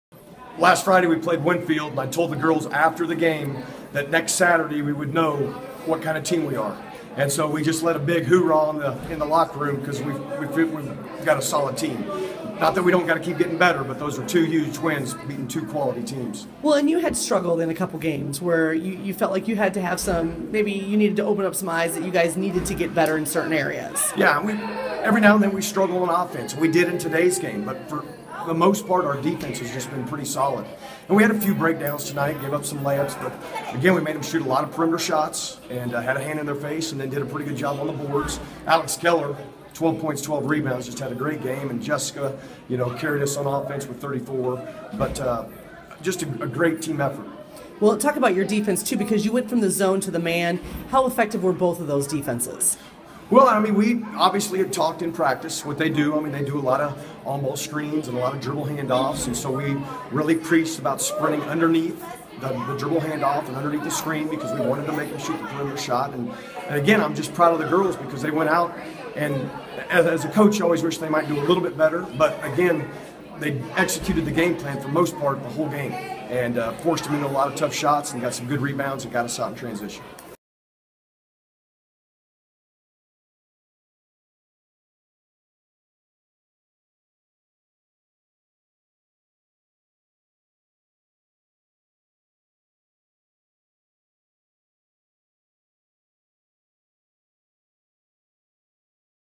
Buhler Girls Coach Jon McLean
Buhler girls coach Jon McLean talks about his team's 53-43 win over Andover on Friday, Jan. 16, 2015 at Buhler.

Video by Joanna Chadwick of the Wichita Eagle and VarsityKansas.com.